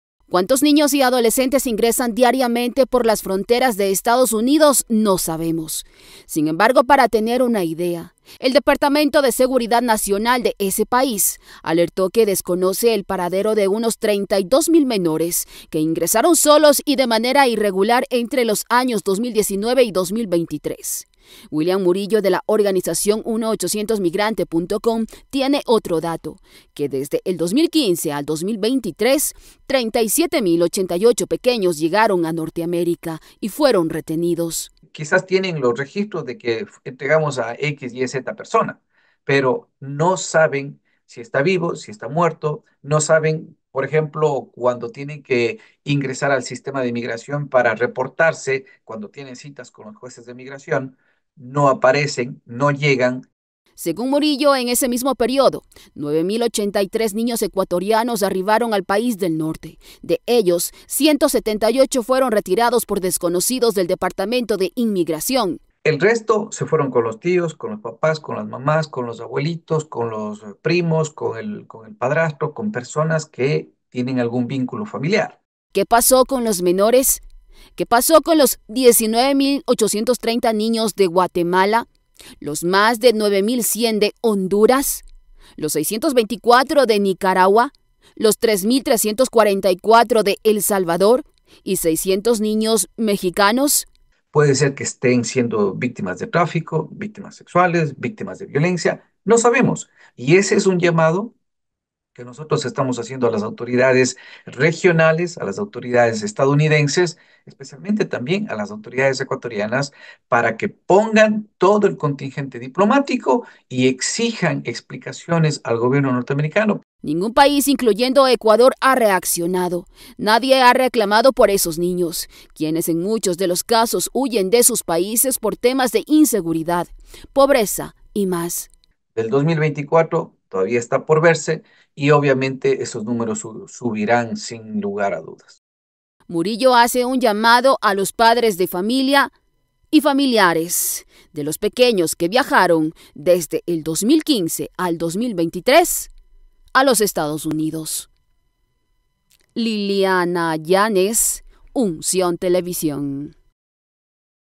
EEUU desconoce el paradero de 32 mil niños, ingresaron por la frontera entre 2019 y 2023
Estados Unidos desconoce el paradero de unos 32 mil niños que ingresaron por las fronteras entre el 2019 y 2023. Entre ellos, se encuentran ecuatorianos y de países de Centroamérica. La organización 1800migrante.com hace un llamado a las autoridades, pues nadie ha reclamado por ellos.